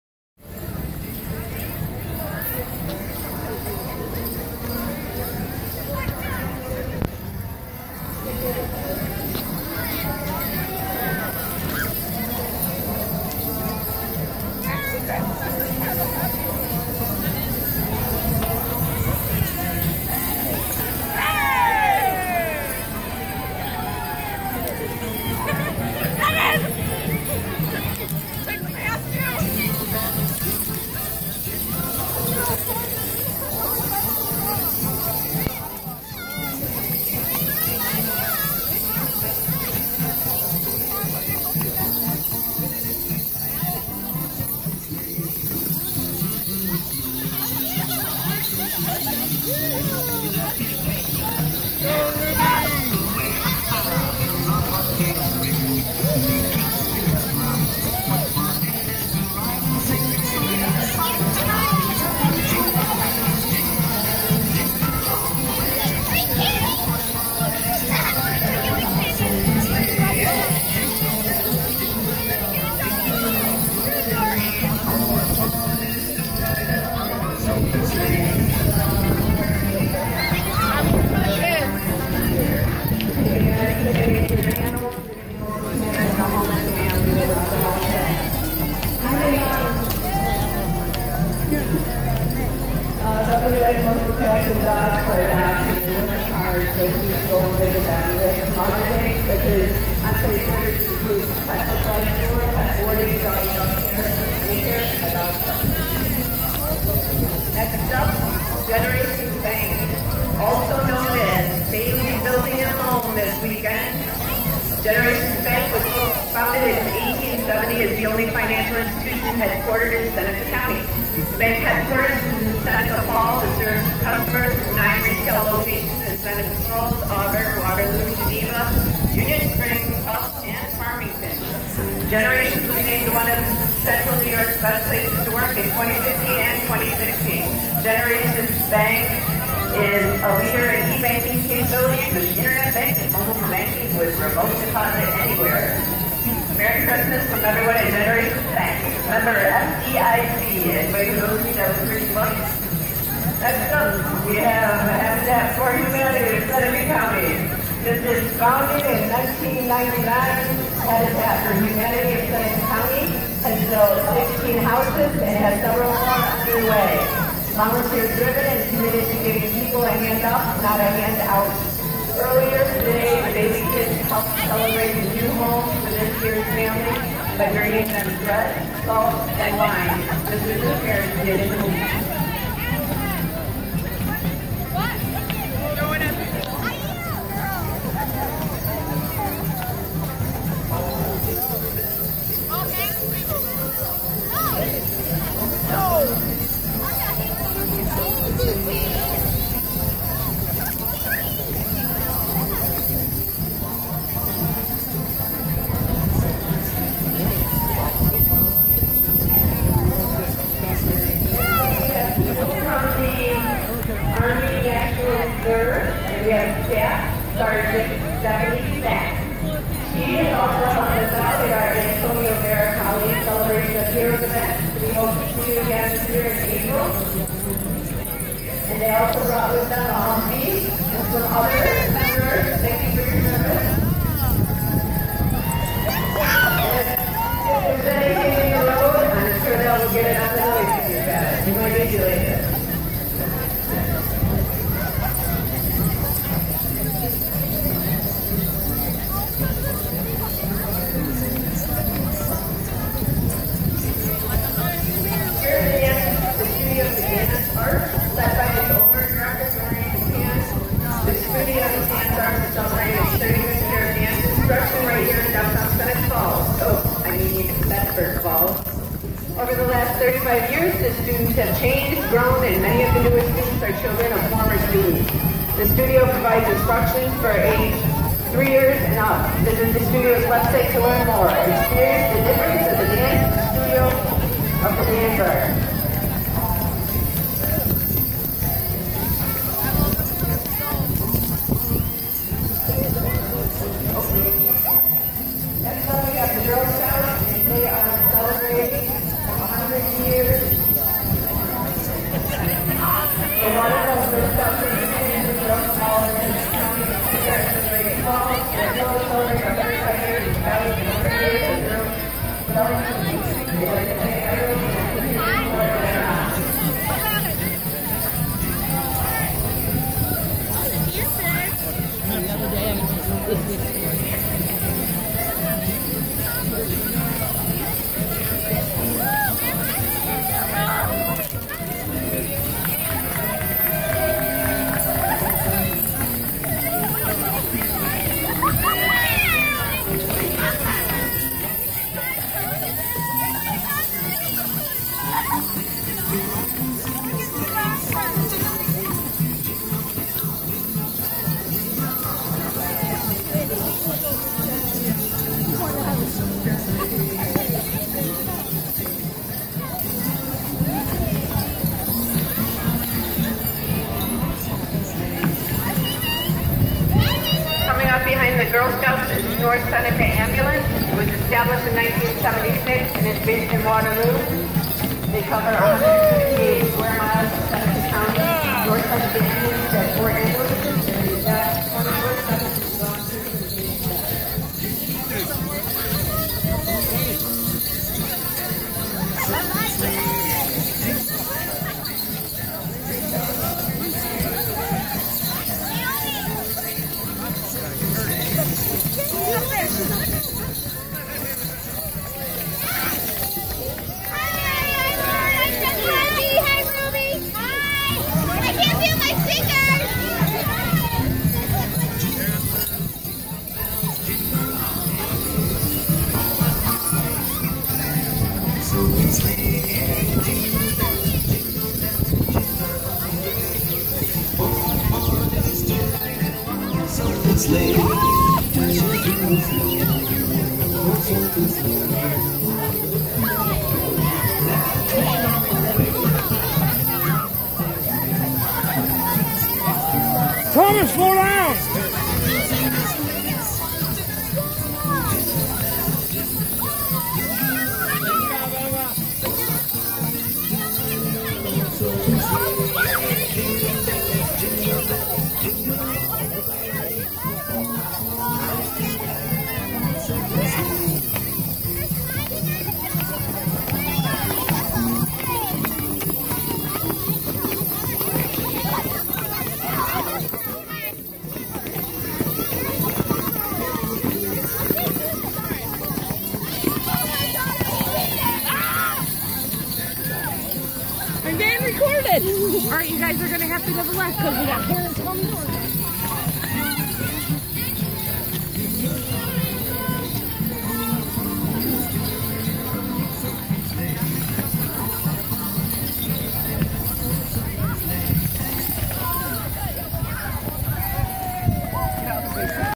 Studio Of The Dance Arts IAWL Parade 12/10/2016
Dancers, Parents, and Friends shook their bells through downtown Bedford (Seneca) Falls to help the Studio celebrate its 35th year and to give a lot of Angels their wings!